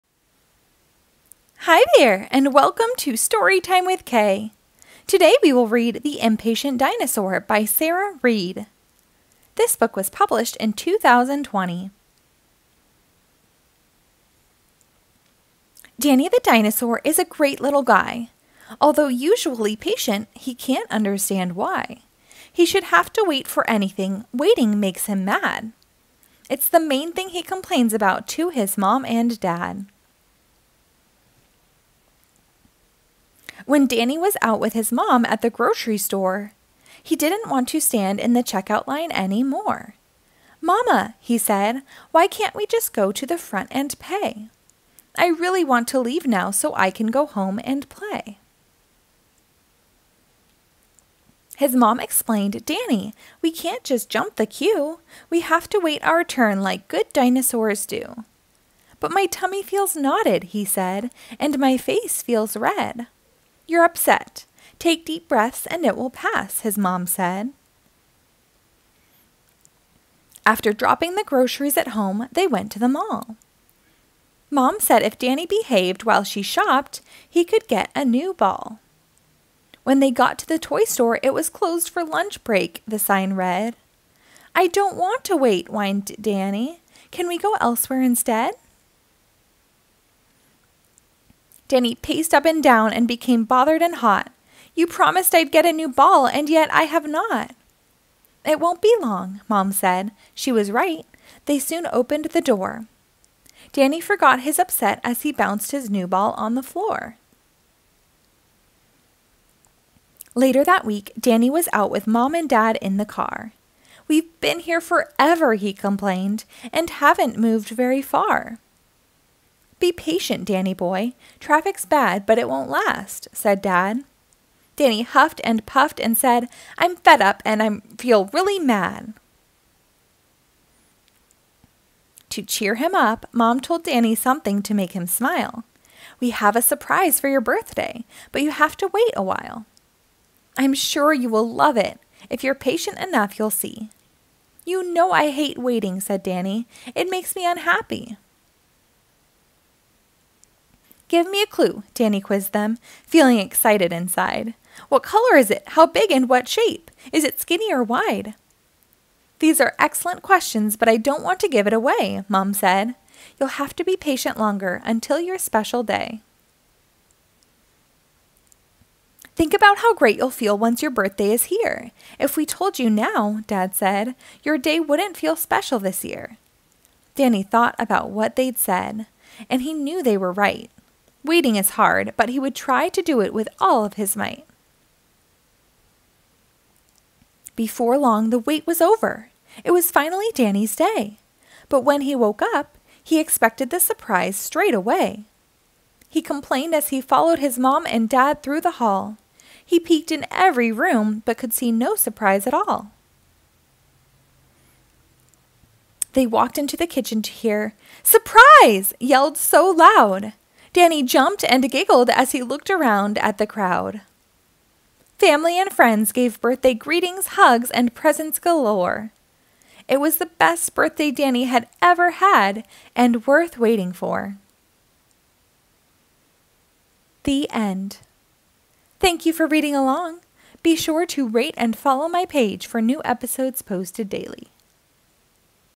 🦖 Kids Book Read Aloud 🦖 The Impatient Dinosaur by Sarah Read [ READ ALONG VIDEO ]
Hi friends! 🦖 Listen and follow along as K reads aloud The Impatient Dinosaur by Sarah Read ( ages 3 - 8 ). Every weekday, we will read aloud a new kids book.

The Impatient Dinosaur was published in 2020. Join us tomorrow to hear a new kids book read aloud by K!